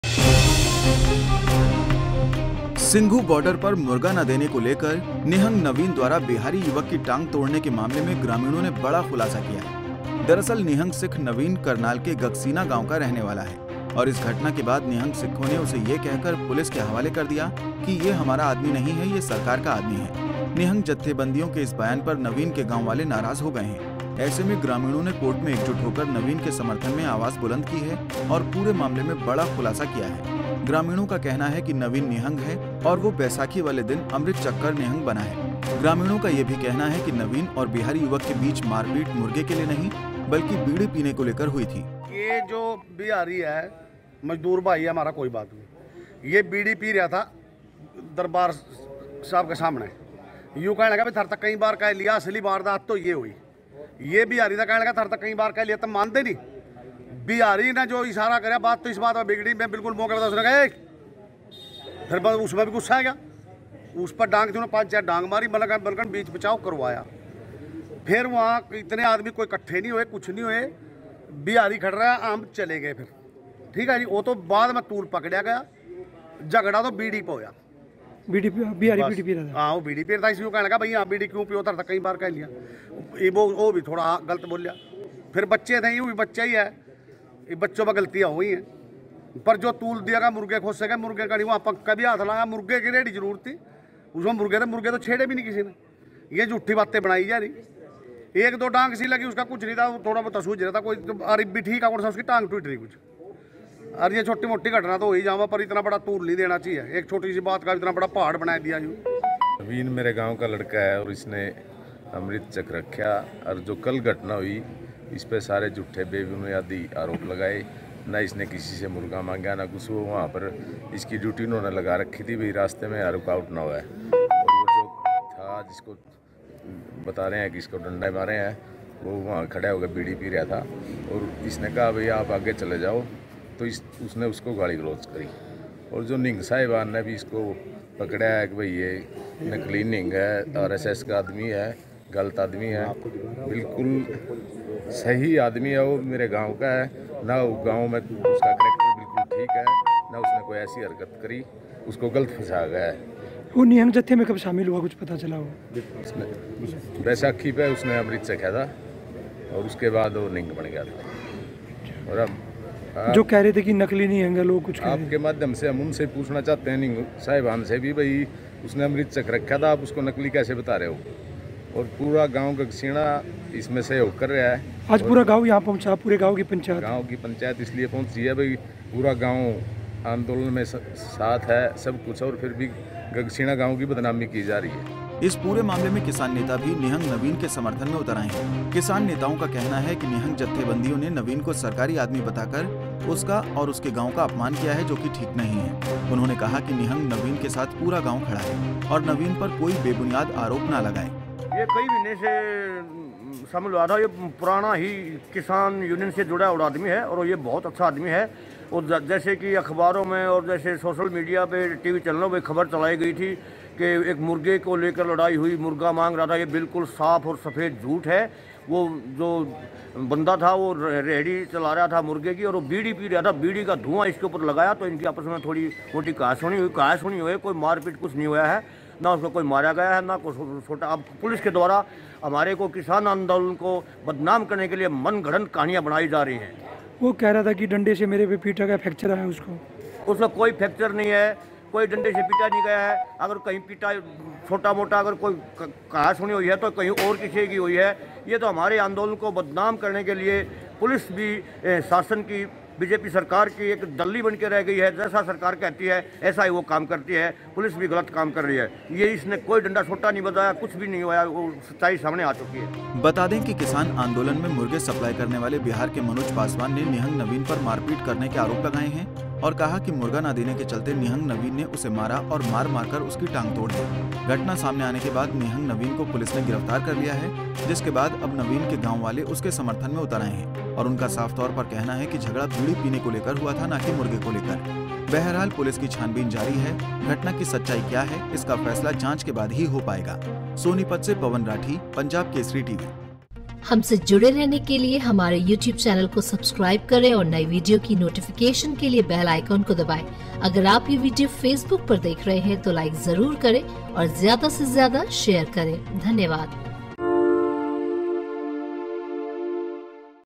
निहंग नवीन के समर्थन में उतरे ग्रामीण, कहा- मुर्गे पर नहीं...बीड़ी को लेकर हुआ था झगड़ा
सिंघु बॉर्डर पर मुर्गा ना देने को लेकर निहंग नवीन द्वारा बिहारी युवक की टांग तोड़ने के मामले में ग्रामीणों ने बड़ा खुलासा किया है. दरअसल, निहंग सिख नवीन करनाल के गगसीना गांव का रहने वाला है, और इस घटना के बाद निहंग सिखों ने उसे ये कहकर पुलिस के हवाले कर दिया कि ये हमारा आदमी नहीं है, ये सरकार का आदमी है. निहंग जत्थेबंदियों के इस बयान पर नवीन के गांव वाले नाराज हो गए हैं, ऐसे में ग्रामीणों ने कोर्ट में एकजुट होकर नवीन के समर्थन में आवाज बुलंद की है और पूरे मामले में बड़ा खुलासा किया है. ग्रामीणों का कहना है कि नवीन निहंग है, और वो बैशाखी वाले दिन अमृत चख कर निहंग बना है. ग्रामीणों का ये भी कहना है कि नवीन और बिहारी युवक के बीच मारपीट मुर्गे के लिए नहीं मारा, बल्कि बीड़ी पीने को लेकर हुई थी.